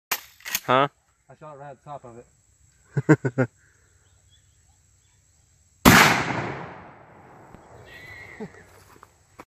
Gun therapy
Trying out a Federal 1600fps 12 guage slug.